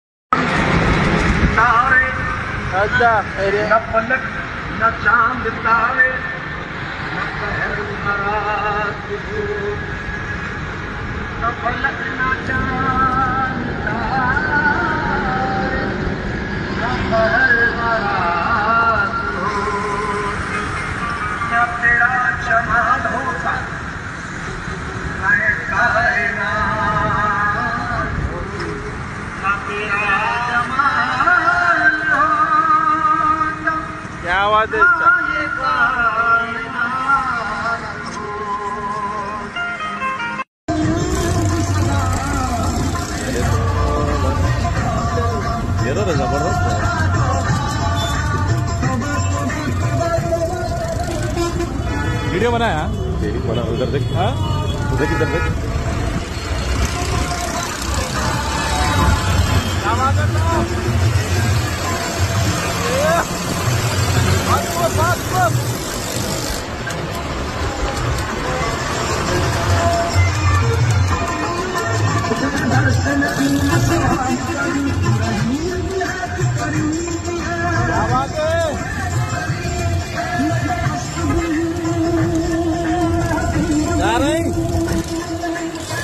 Chacha Ji Iss Umr meh aur Iss Halaath meh bhi Bahoot Maaze meh Auto Chalateh Hue Nazar Aaye Hyd meh
Chacha Ji Iss Umr meh aur Iss Halaath meh bhi Bahoot Maaze meh Auto Chalateh Hue Nazar Aaye Hyderabad meh

ALLAH, Hum Saab ko Lambi Umr aur Achi Sehet Atta Farma